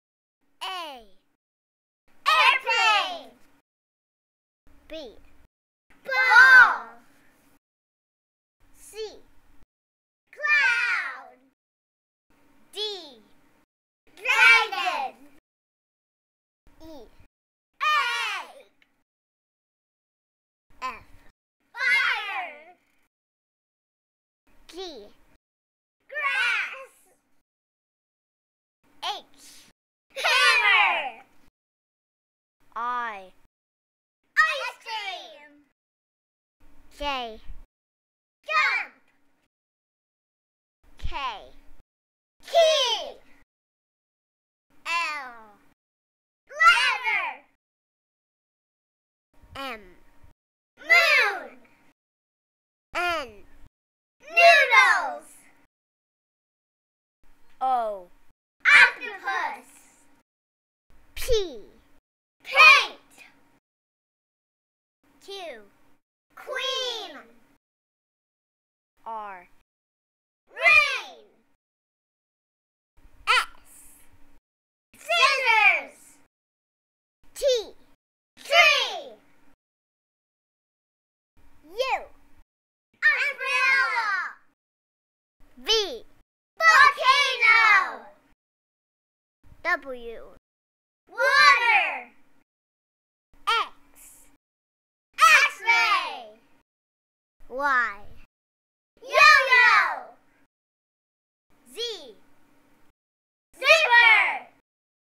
3D Alphabet
This is a film about the English alphabet, created with the help of my children who provided the audio. Each letter is shown and said, and we see the letter turn into something that begins with that letter.

A - Airplane
B - Ball
C - Cloud
D - Dragon
E - Egg
F - Fire
G - Grass
H - Hammer
I - Ice Cream
J - Jump
K - Key
L - Ladder
M - Moon
N - Noodles
O - Octopus
P - Paint
Q - Queen
R - Rain
S - Scissors
T - Tree
U - Umbrella
V - Volcano
W - Water
X - X-ray
Y - Yo yo
Z - Zipper

Each letter comes to life in 3D animation! What are YOUR favorite letters?